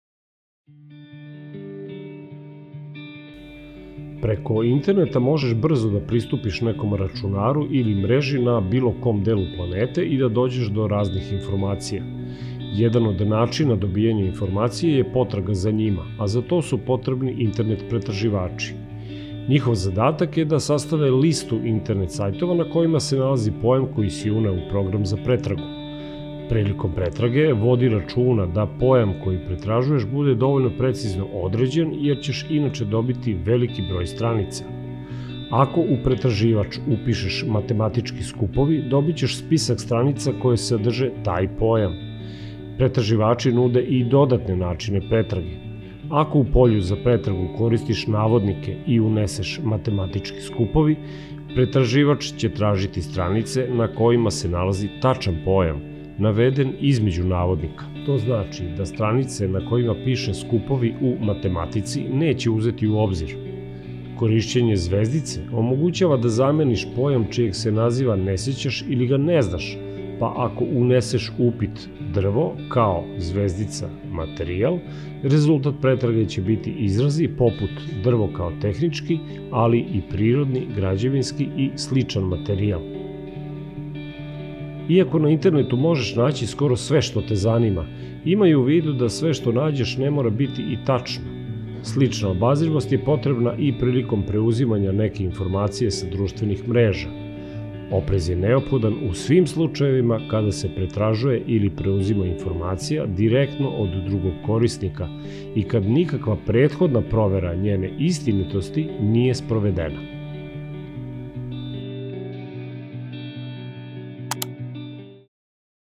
Internet pretraga
📚 Peti razred - Tehnika i tehnologija
📝 Oblast: Tehnička i digitalna pismenost
📌 Lekcija: Internet pretraga i pristup online resursima

💡 Preko interneta možeš brzo da pristupiš nekom računaru ili mreži sa bilo kojeg dela planete.